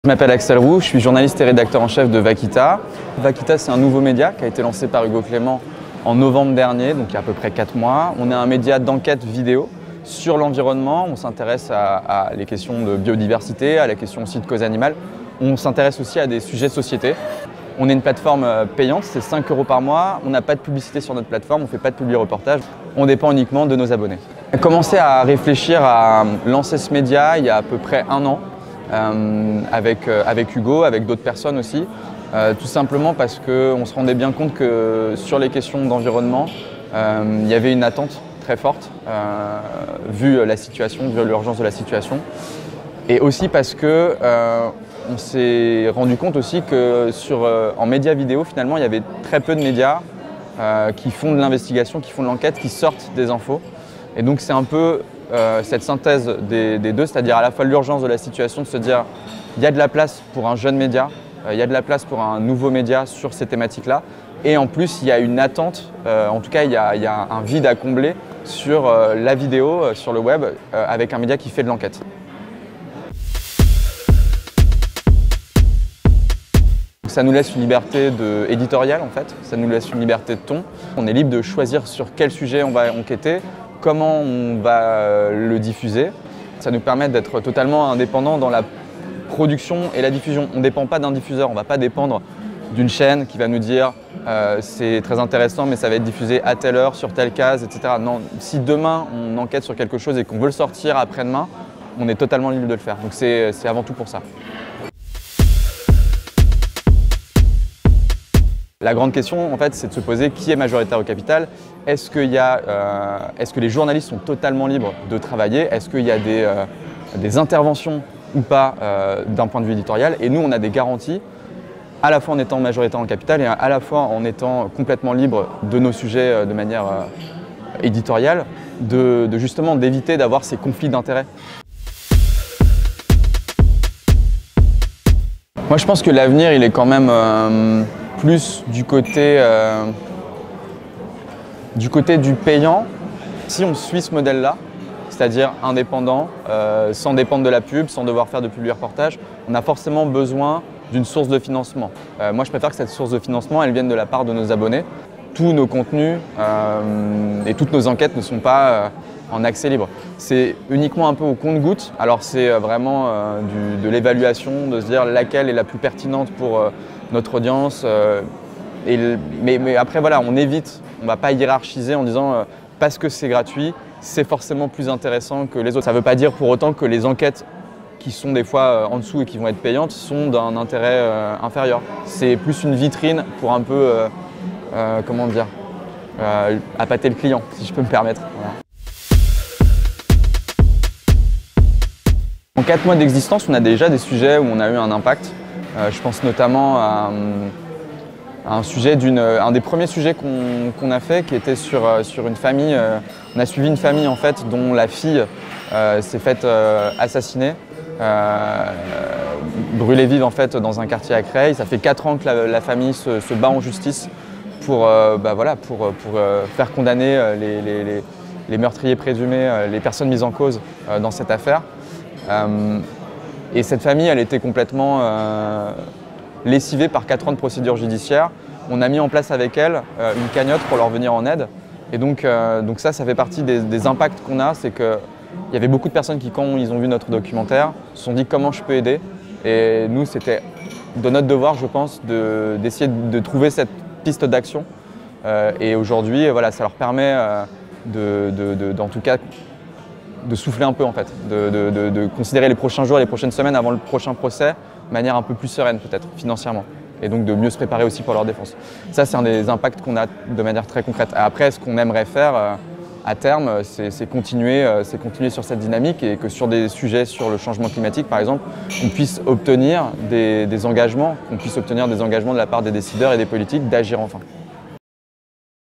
4 questions à Axel Roux - Assises du Journalisme 2023
Axel Roux est rédacteur en chef de Vakita. Créé en novembre 2022, ce média d'enquête et d'action se spécialise sur les sujets de société et d'environnement.